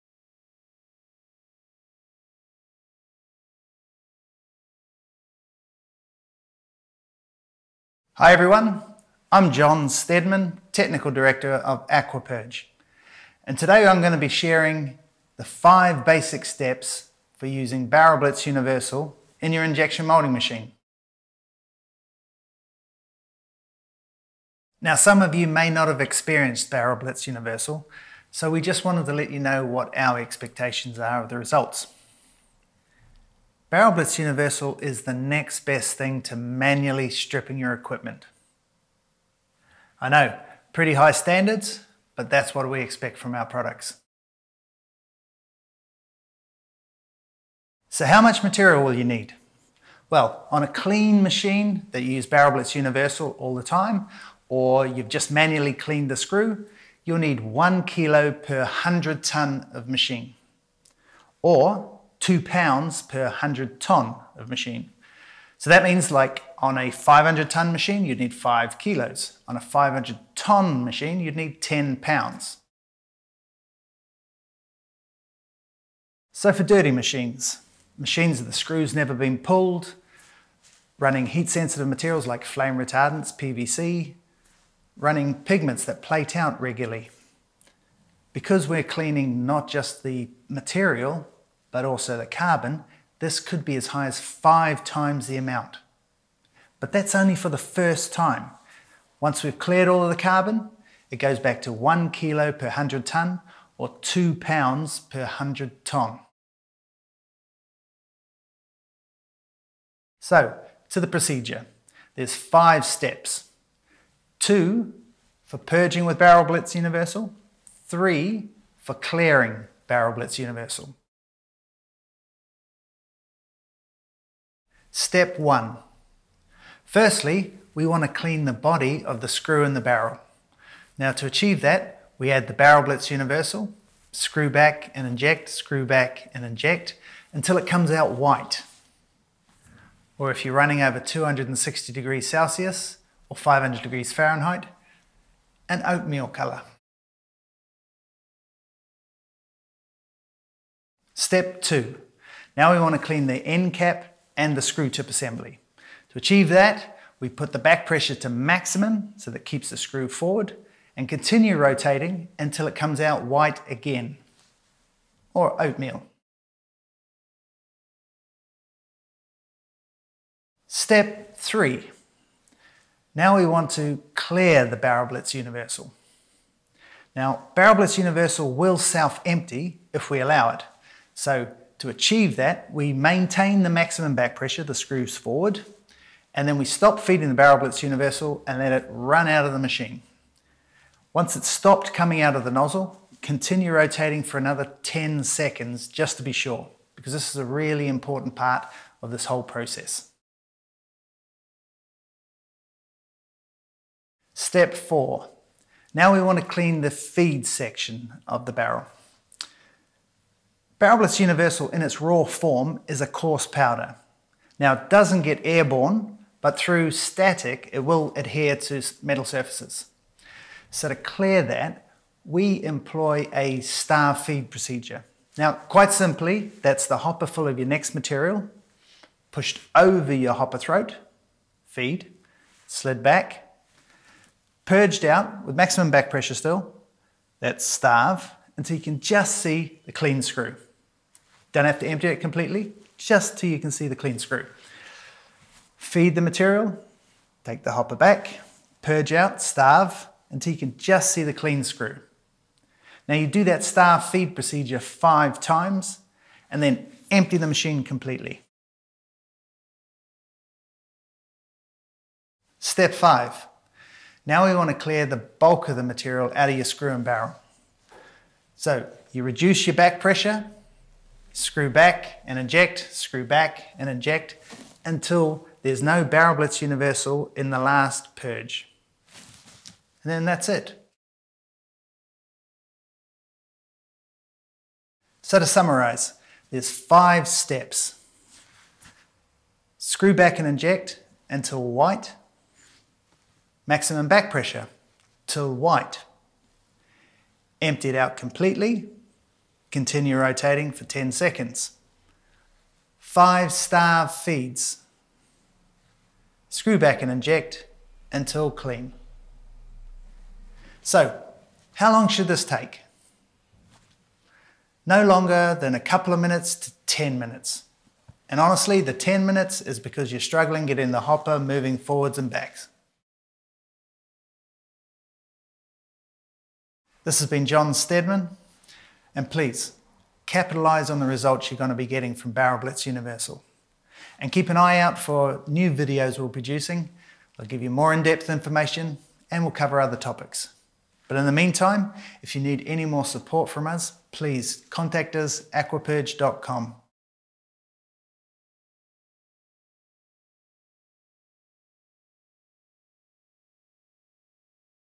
Barrel Blitz Universal - Instructions for Injection Moulding
Our Technical Director John Steadman runs you through how to use Aquapurge's flagship product, Barrel Blitz Universal (BBU).